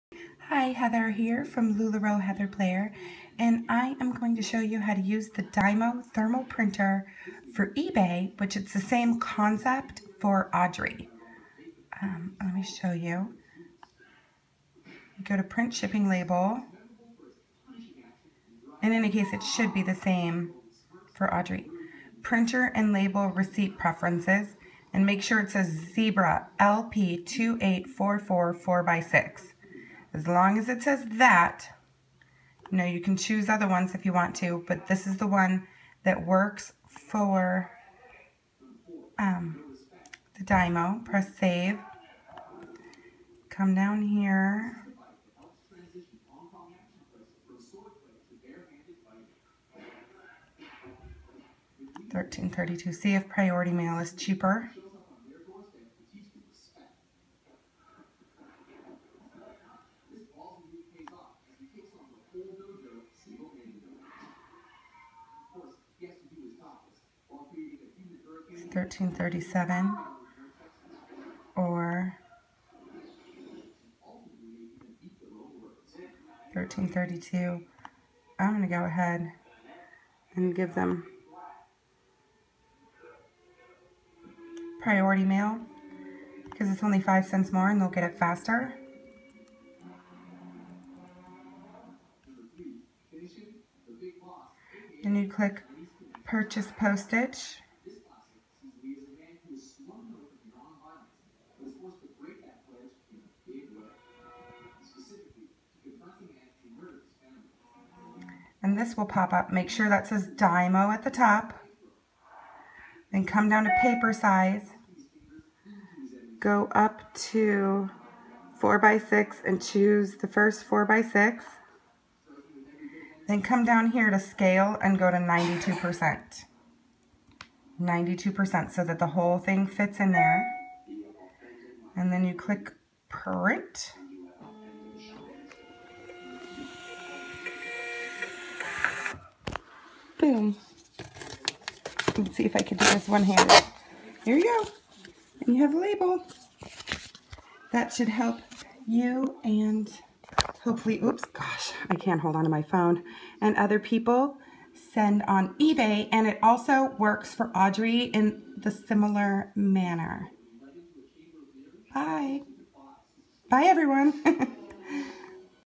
How to use the DYMO LabelWriter 4XL To Print Shipping Labels For LuLaRoe Audrey And eBay On Mac Book
Join my LuLaRoe Team! If you would love to become a LuLaRoe Consultant and be on my team,